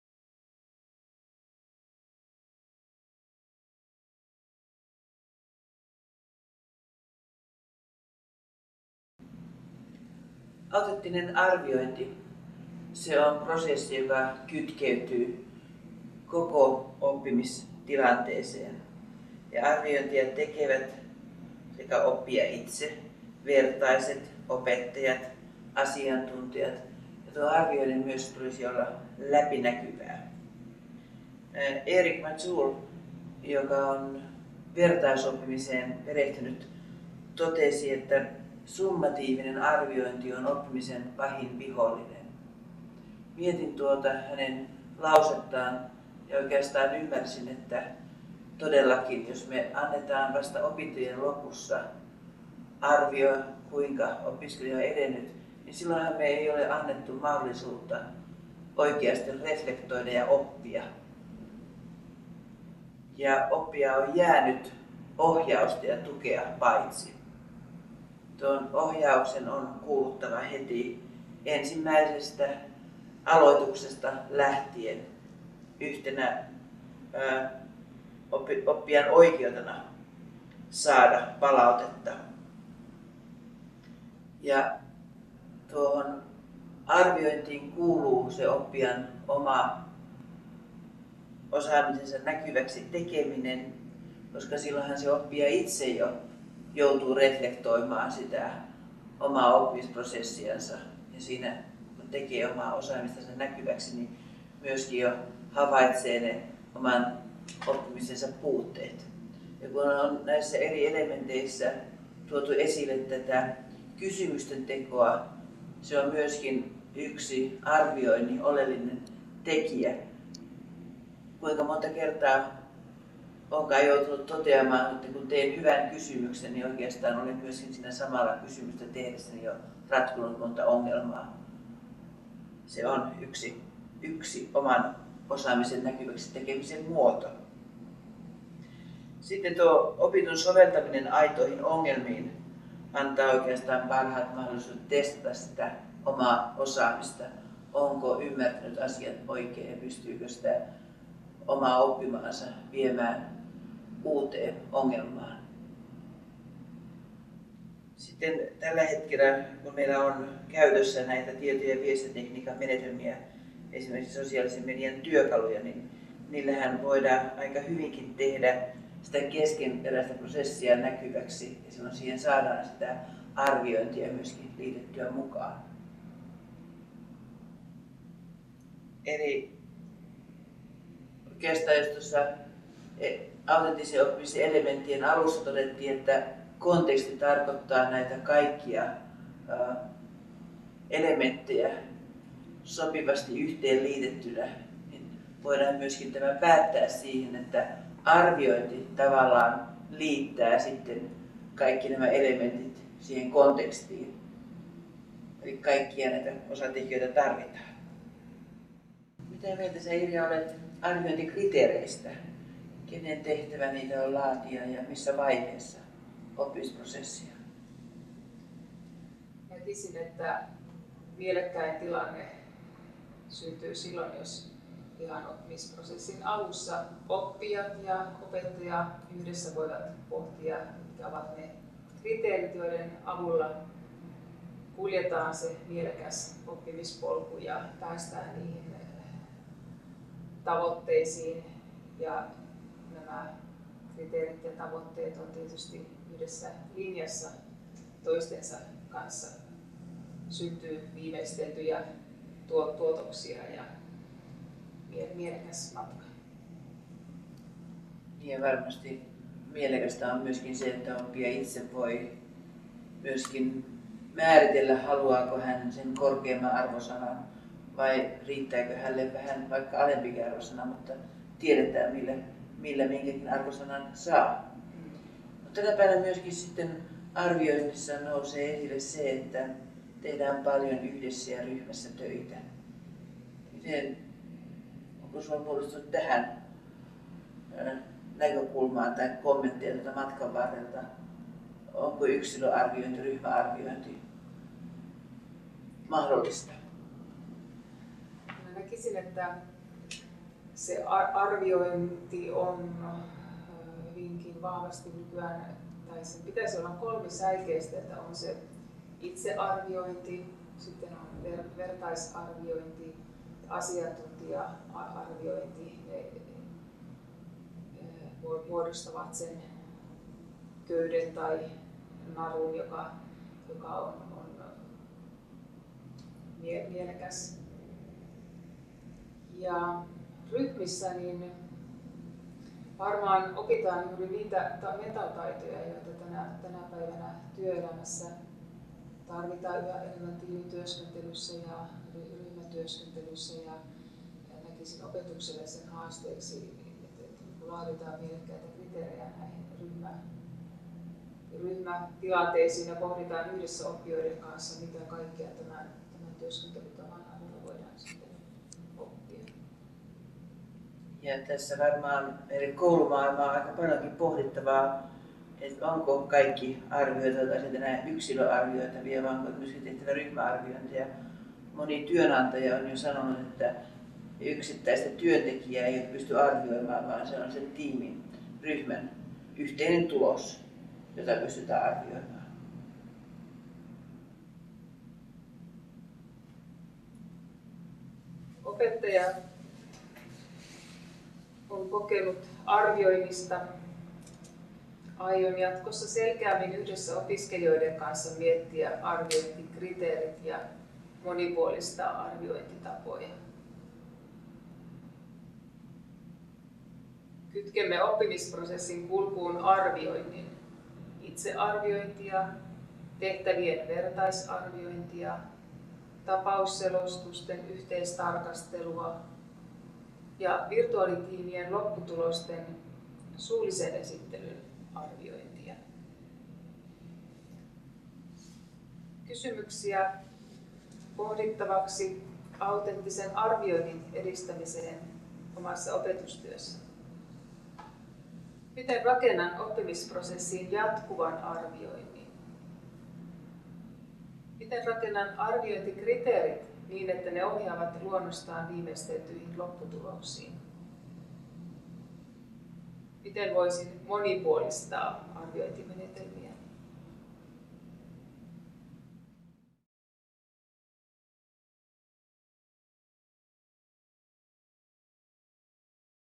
9. Autenttinen arviointi
Tämä yhdeksäs video autenttisesta oppimisesta. Tässä videossa käsitellään autenttista arviointia ja annetaan esimerkkejä sen soveltamisesta käytännön opetus- ja oppimistilanteisiin.

Autenttisen oppimisen -videosarjassa kuvataan yhdeksän elementin avulla autenttisen oppimisen lähestymistapaa, jota voidaan käyttää oppimisympäristöjen suunnitteluun ja kehittämiseen erilaisissa koulutuskonteksteissa.

Asiantuntijat: Irja Leppisaari, Centria ammattikorkeakoulu. Leena Vainio, Hämeen ammattikorkeakoulu. Ayvot-hanke ja OpenPäivitys 2012. Toteutus Suomen VirtuaaliAMK/Ayvot.